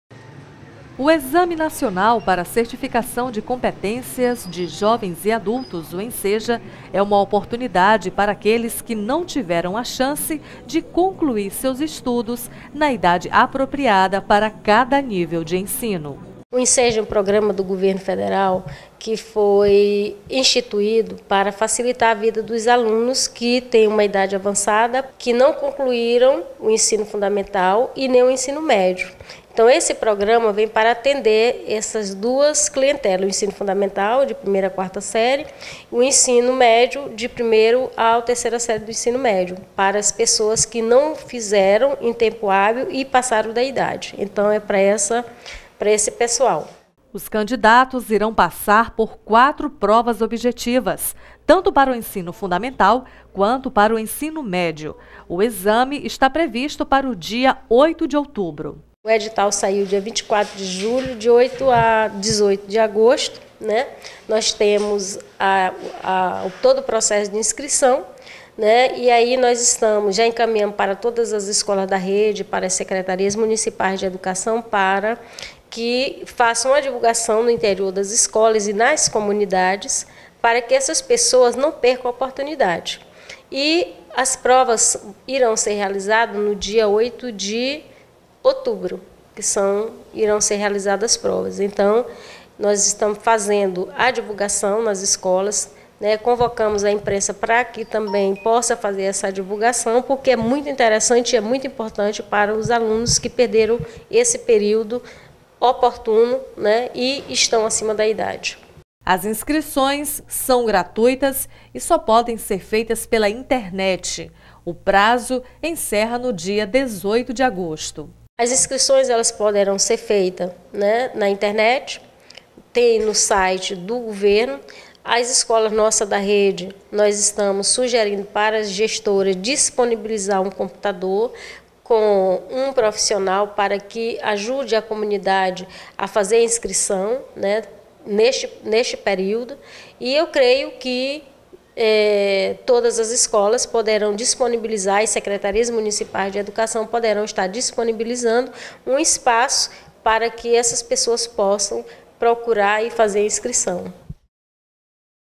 TV Remanso - ENCCEJA- 2017
Acompanhe nessa entrevista as orientações que a professora Maria Zuila, gestora da URE, deu à nossa equipe sobre o ENCCEJA 2017 - Exame Nacional para Certificação de Competências . Tire aqui suas dúvidas e conclua seus estudos, a oportunidade é única e valiosa.